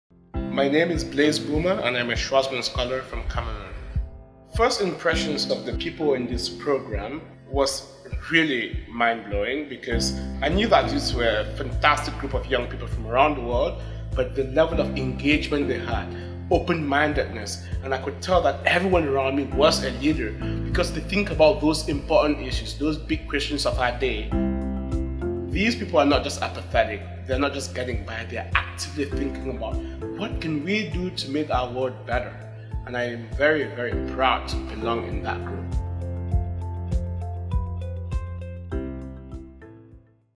Spotlight on a Scholar: Blaise Buma on Meeting His Fellow Scholars
With the year now underway, we are excited to share insights from the Scholars in their own words.

Blaise Buma is a recent graduate of Washington and Lee University and currently works at Goldman Sachs, which has given him leave to join the first class of Schwarzman Scholars. He grew up in Cameroon and from a young age set his sights on pursuing higher education abroad. Blaise is the co-founder of Open Dreams, an international education NGO. As a Schwarzman Scholar, Blaise plans to study China’s economic “miracle” and to use this knowledge to help shape the future trajectory of his home country. Blaise is 28 years old and from Cameroon.